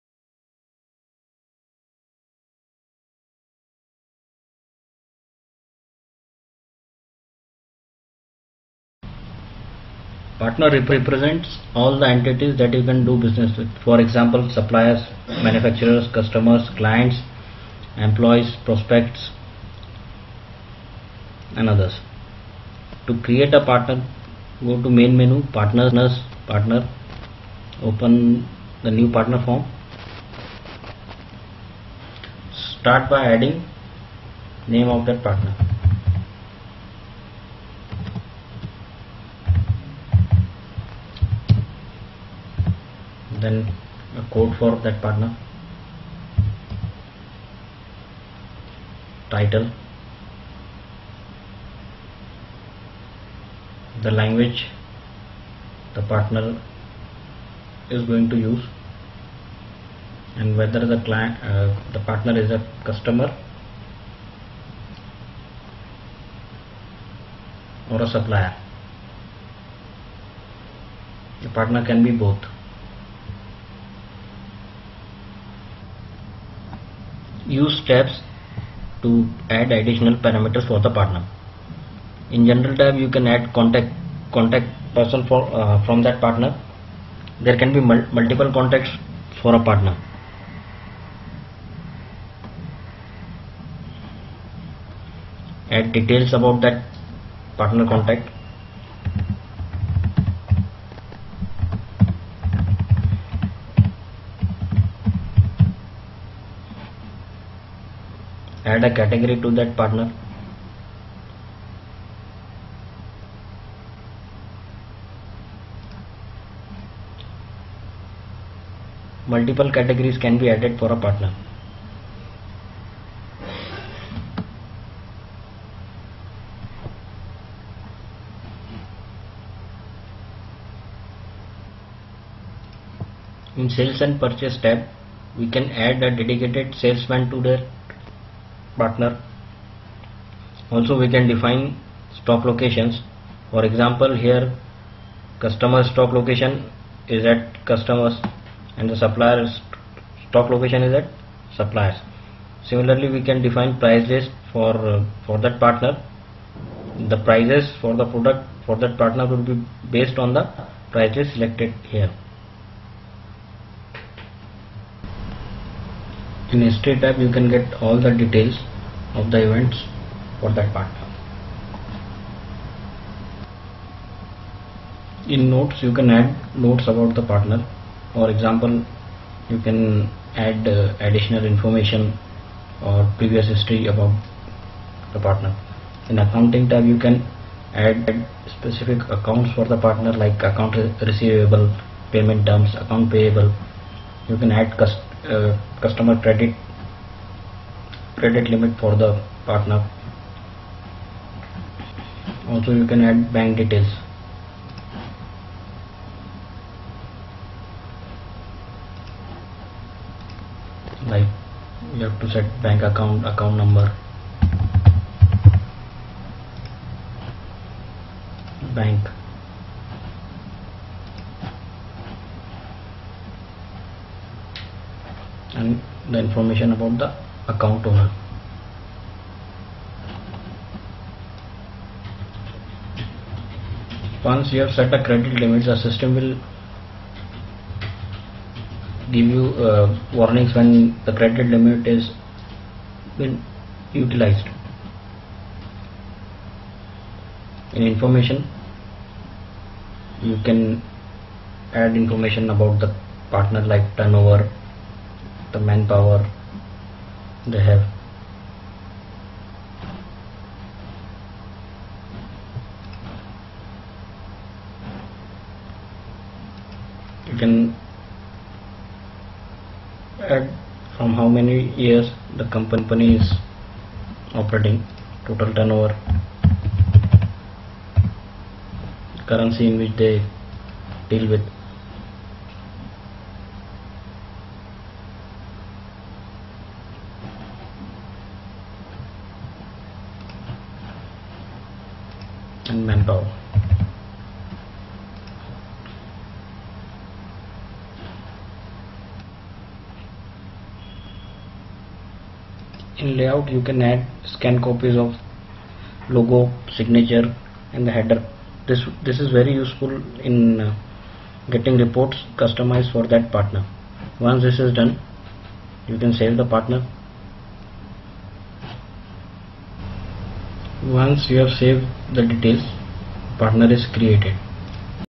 Odoo OpenERP Partner Management
Pragmatic Technology Solutions Software Company Provideing solutions for cutting edge technologies in Odoo OpenERP, Ruby on Rails, Talend, Jaspersoft, Twilio, OpenVBX, CRM, CMS. This is the screencast of Partner management in Odoo OpenERP